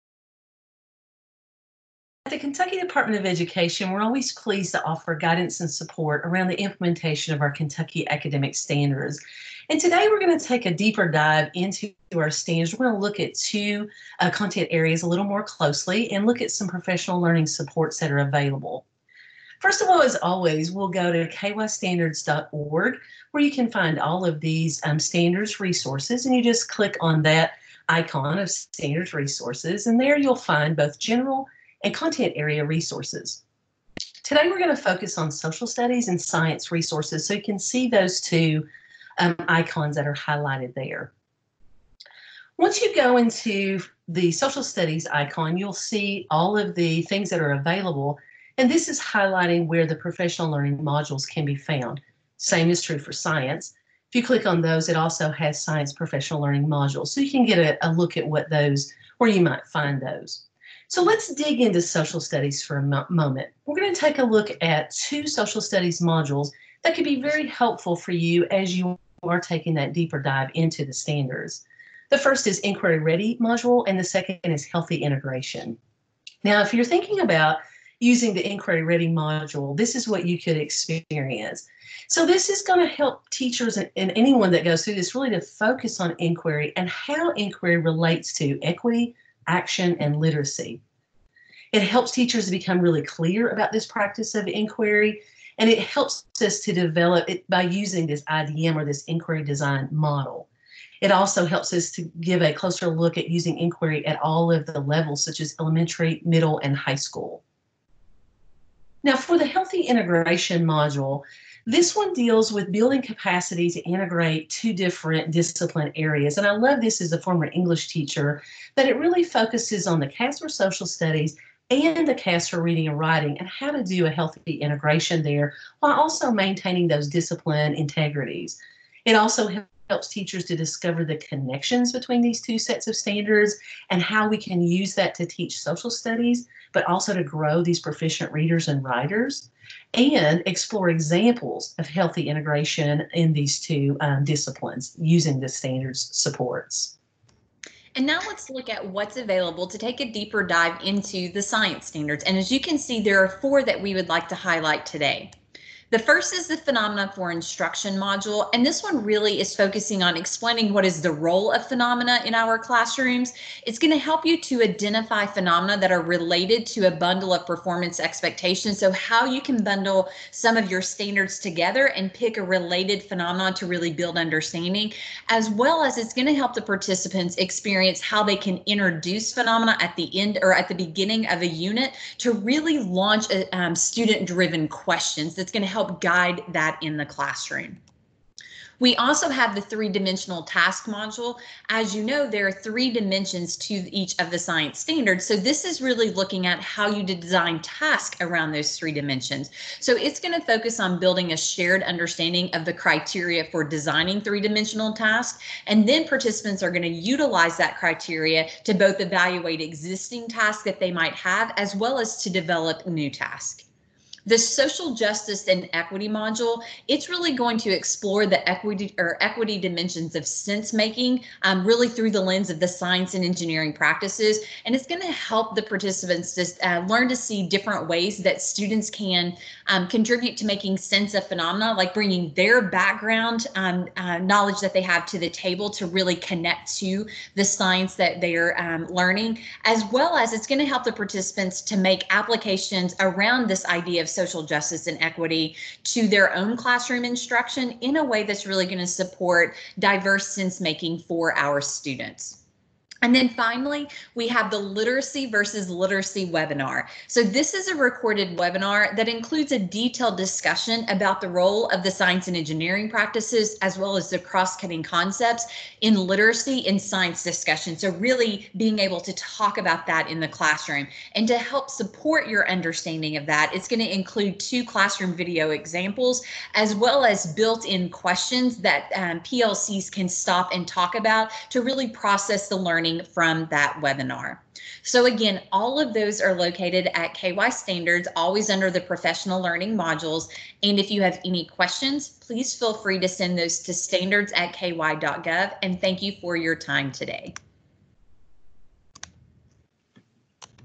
KDE - PL Support Video - Deeper Dive Into the Standards for Social Studies and Science
Having a deep understanding of the Kentucky Academic Standards is the foundation for building strong instruction. This short video highlights modules from both Social Studies and Science to help educators navigate a deeper dive into the content standards.